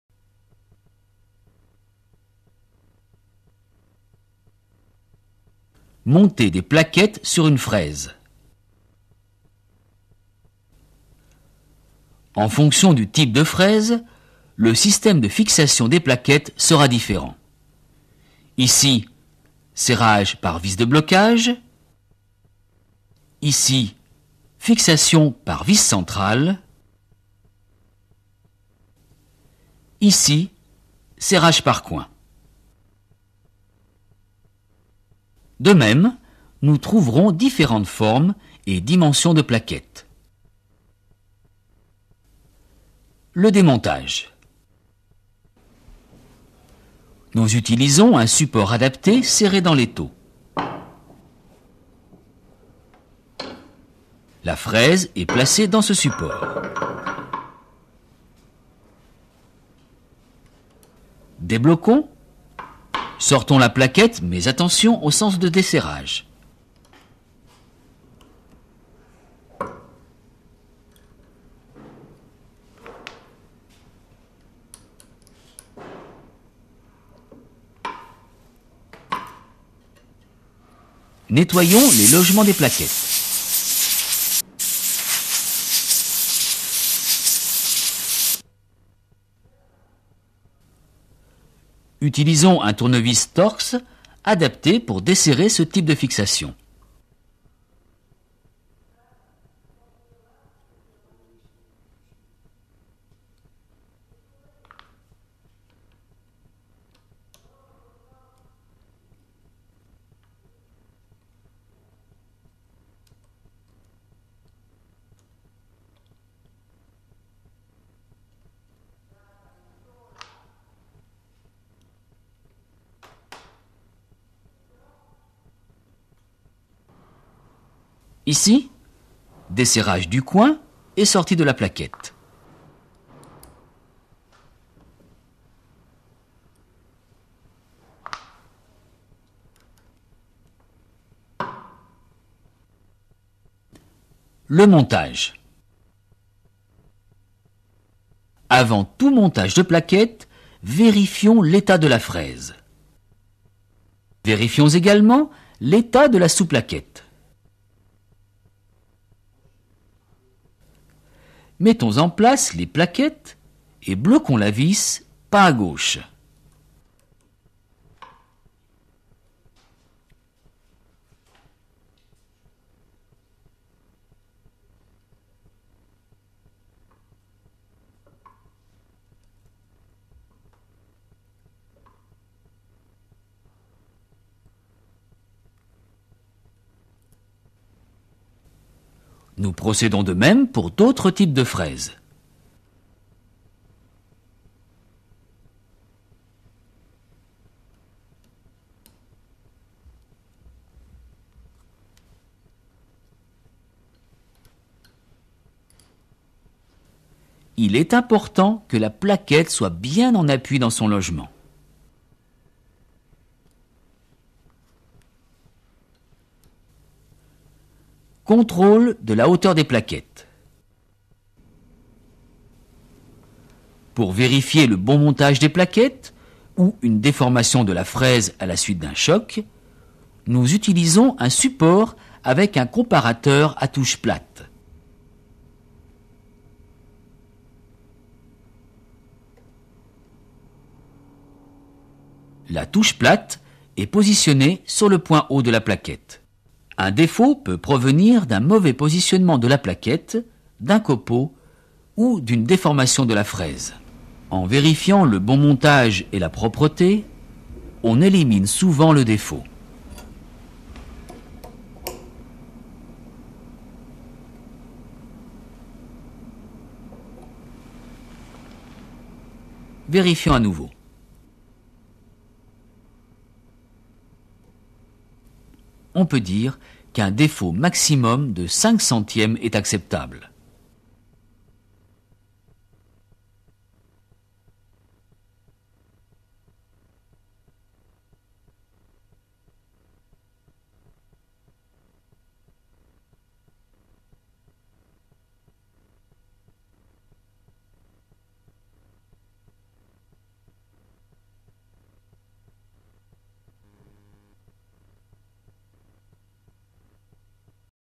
Usinage - Monter des plaquettes sur une fraise
Cette vidéo issue des archives (1996) du département sectoriel industrie de l'AFPA, elle  fait partie d'une série sur l'usinage. le film montre comment monter des plaquettes sur une fraise